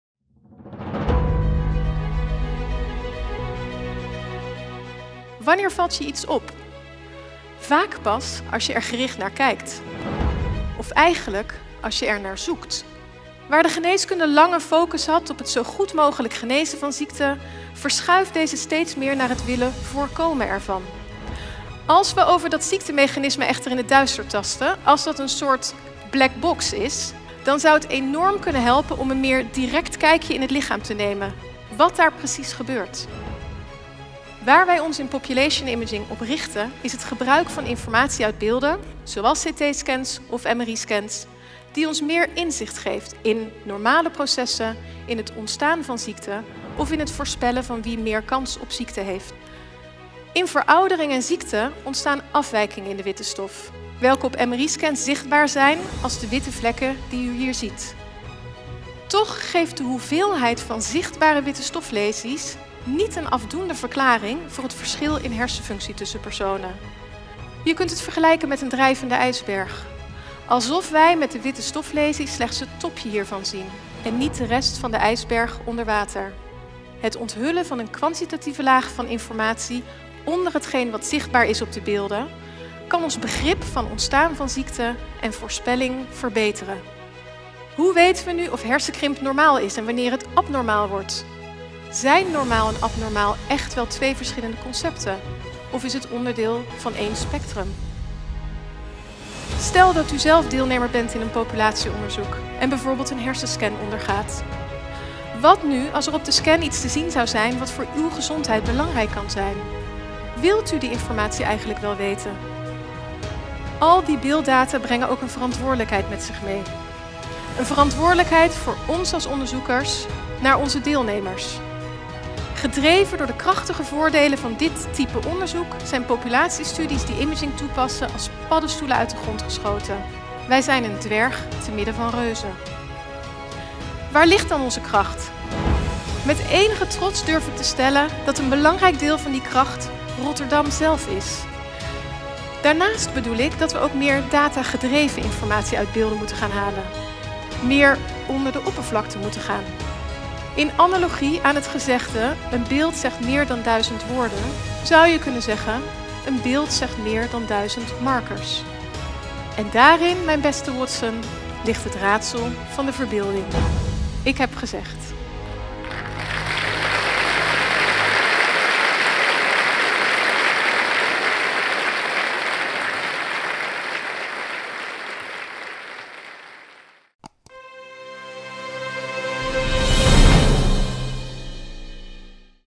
Sherlock en het raadsel van de verbeelding. Oratie Prof. dr. Meike Vernooij (trailer)
“The world is full of obvious things which nobody by any chance ever observes.”
De leerstoel van professor Meike Vernooij draait om observeren. Hierbij gaat het zowel over het herkennen van datgene wat voor de hand ligt, “the obvious” zoals in het citaat van Sherlock Holmes hierboven, maar juist ook over hetgeen we niet zien als we er niet goed naar zoeken, en waar zelfs enige verbeeldingskracht voor nodig is. Prof. Meike Vernooij bespreekt in haar oratie op 7 december 2018 hoe het gebruik van beeldvormende technieken in populatiestudies, en de bewerking van deze beelden, een krachtig instrument kan zijn om meer te begrijpen over het ontstaan van ziekte en de voorspelling hiervan.
Inaugurele rede uitgesproken op 7 December 2018